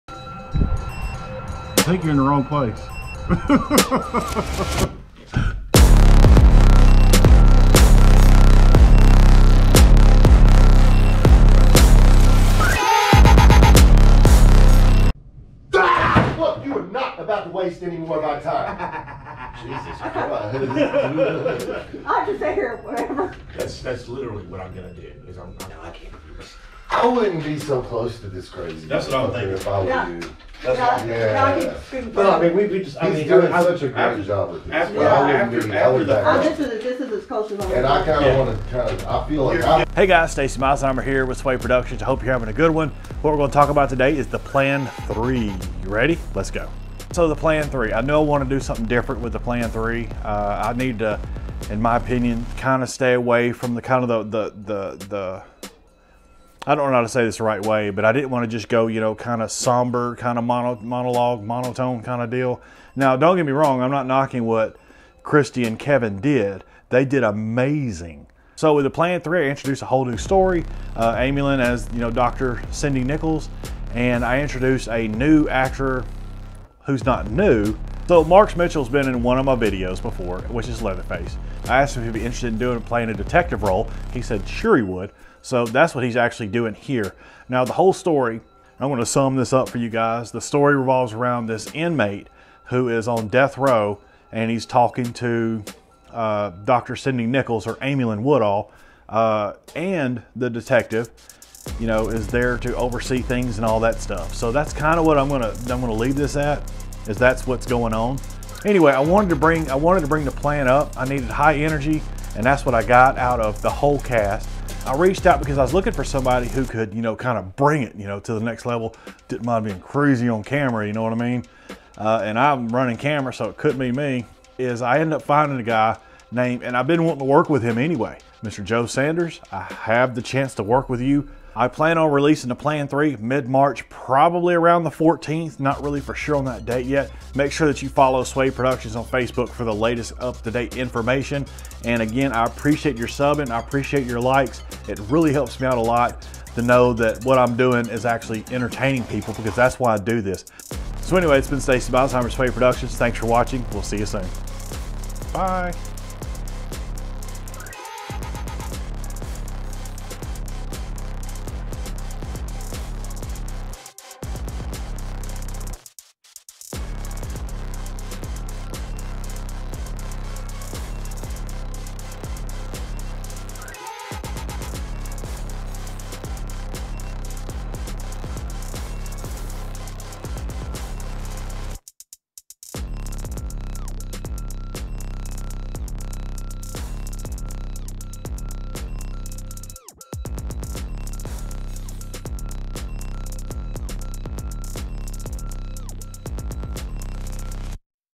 The Plan III BTS and WHEN YOU CAN SEE IT
When can you expect to see this short film?  Find out in this video!
For up to the minute info...check here and give it a like huh?